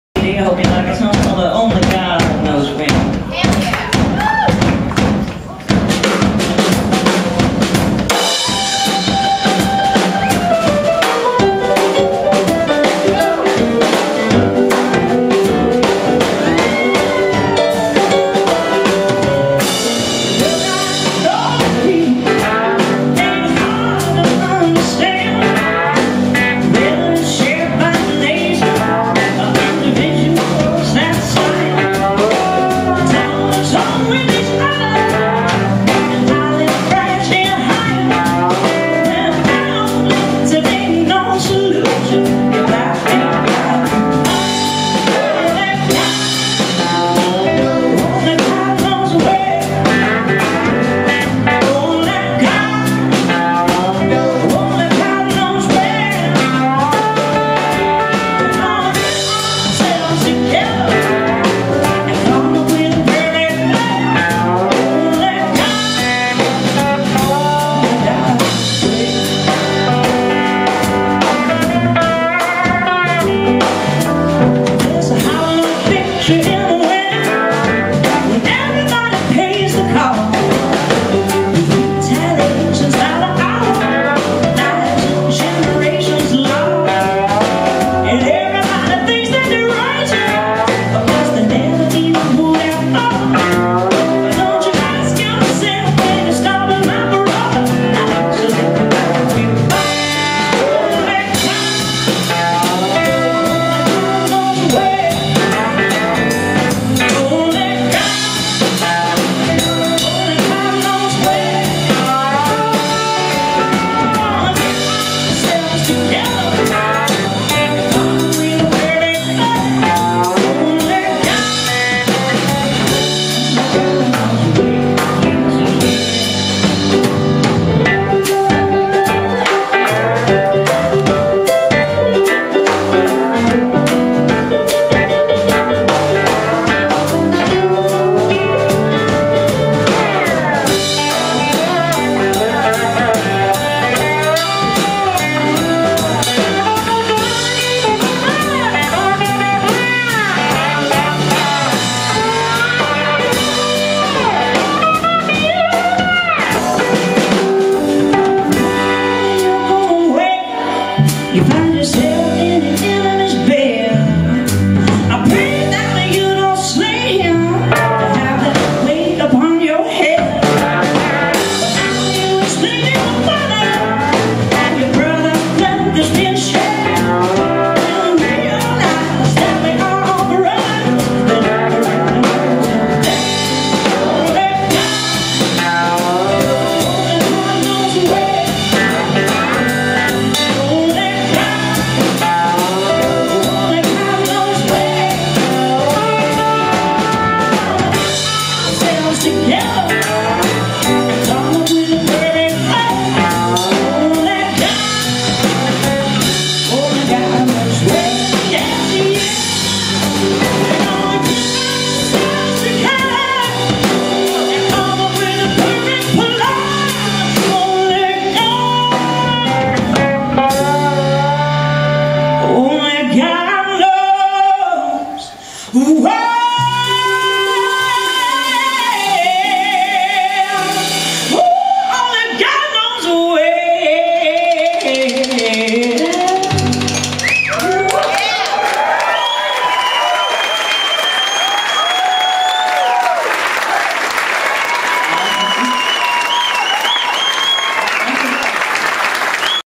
Carolyn Wonderland in Dallas, Texas
Recorded live in June of 2012 at the historic Kessler Theater in X+/North Oak Cliff.